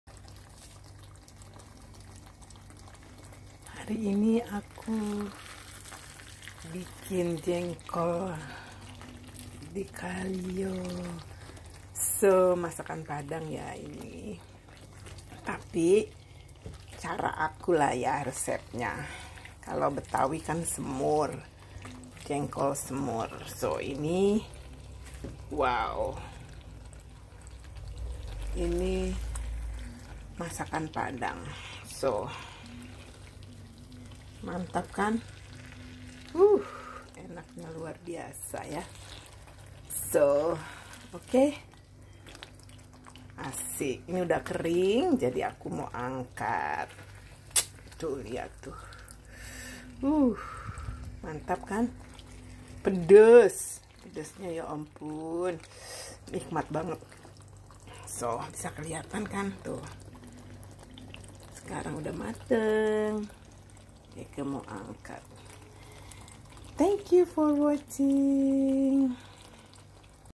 Jengkol Kalio one of Indonesian Dishes
Not many peoples love this foods and can cook them.. I do love and I can cook them with some of different recipes..  Not many peoples can do cook those food...